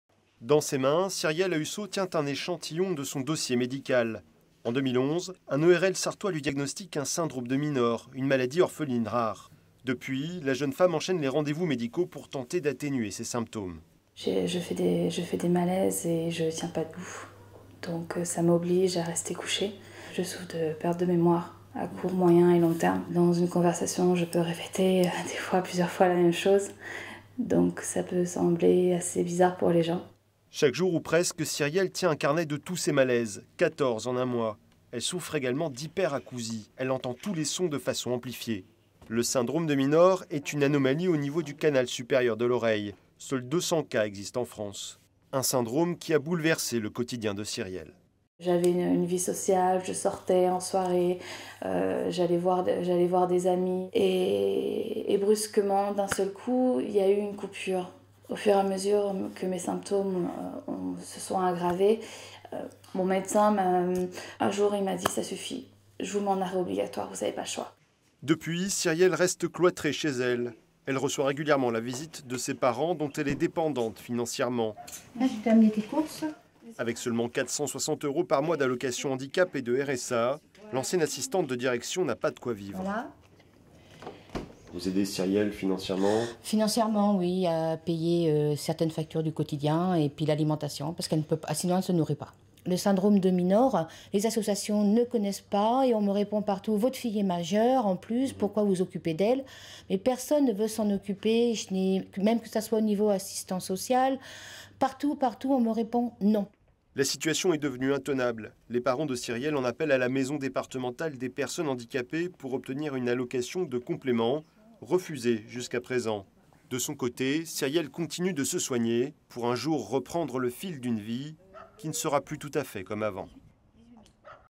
Syndrome de Minor : elle lutte pour faire reconnaître sa maladie (Sarthe)
Elle souffre d'une maladie orpheline rare, mais la société ne reconnaît pas son handicap.

Cyrielle Hahusseau est atteinte du syndrôme de Minor. Seuls 200 cas en France sont répertoriés.
Une maladie qui l'empêche de travailler et de vivre normalement. Cyrielle connaît en plus la double peine d'être atteinte d'une maladie rare, non reconnue.
Florent Clavel a recueilli son témoignage.